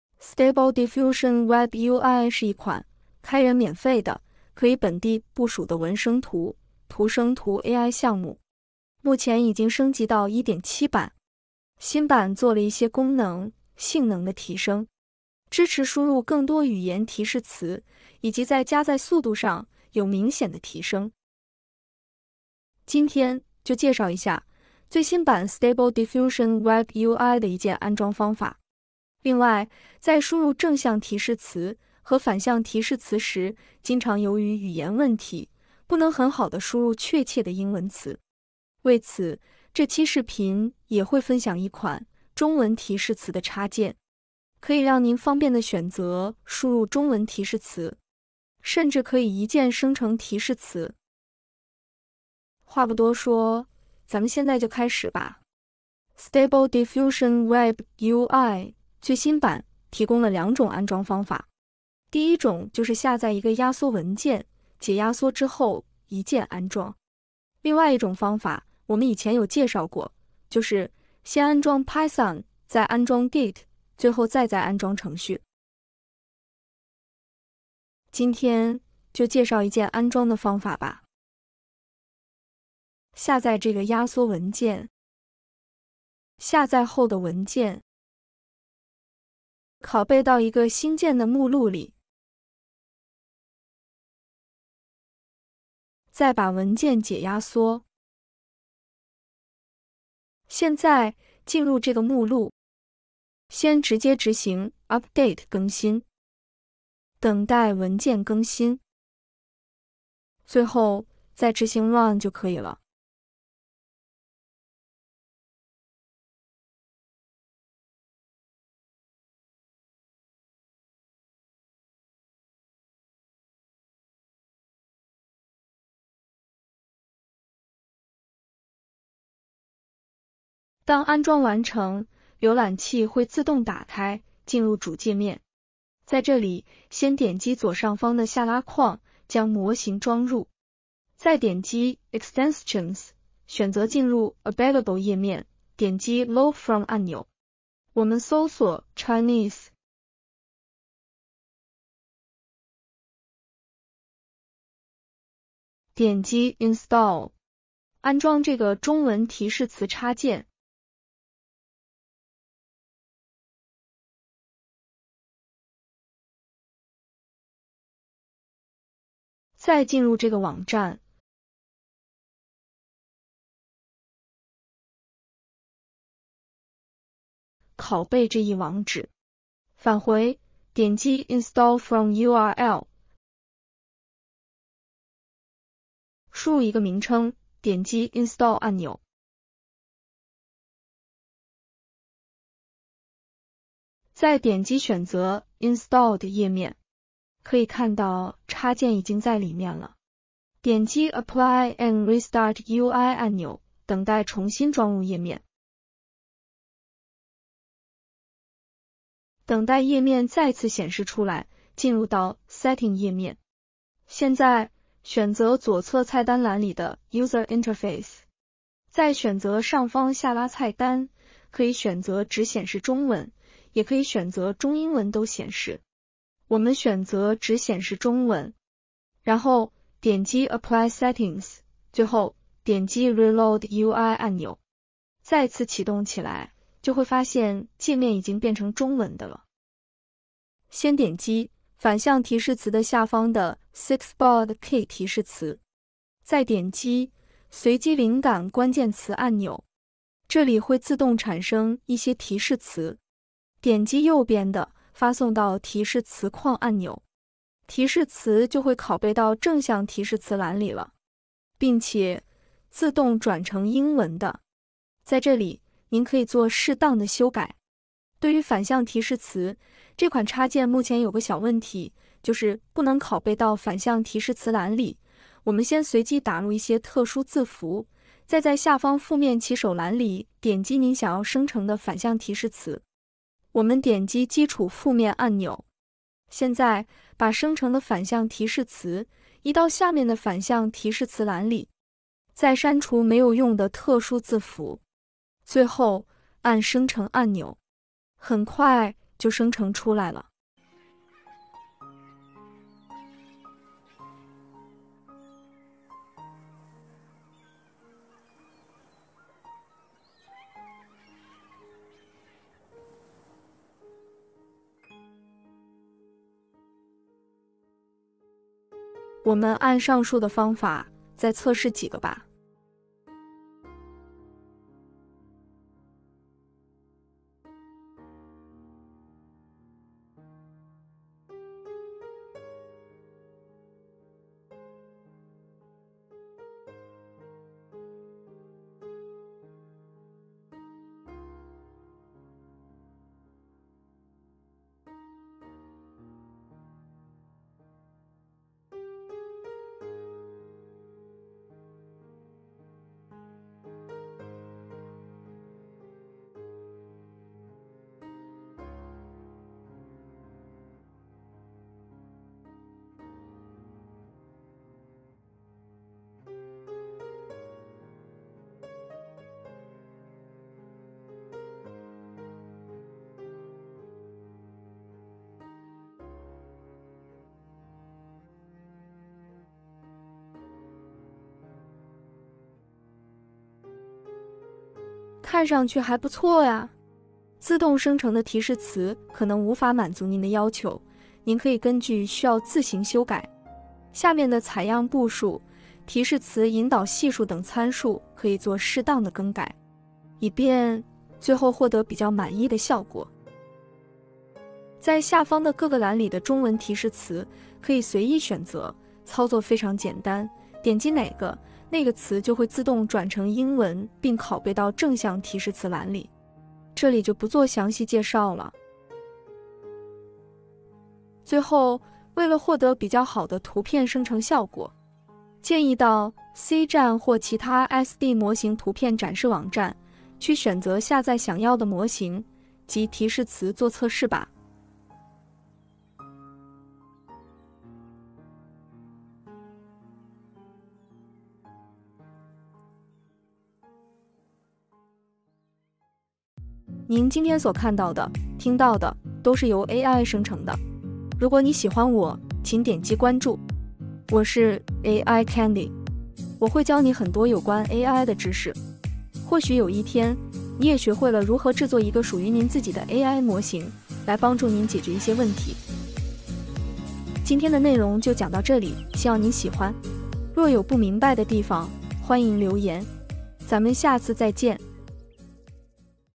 【Stable Diffusion】最新版 WebUI 1.7本地部署, 中文界面，提示词一键生成。
【Stable Diffusion WebUI v1.7】最简单的中文提示词生成插件，让SD WebUI使用更方便。heygen

本视频使用了 AI 工具 HeyGen.

 【Runway】Gen-2 文字转视频一键生成

【AI虚拟主播制作】选择自己喜欢的头像，3分钟学会照片转动态语音播报的方法。heygen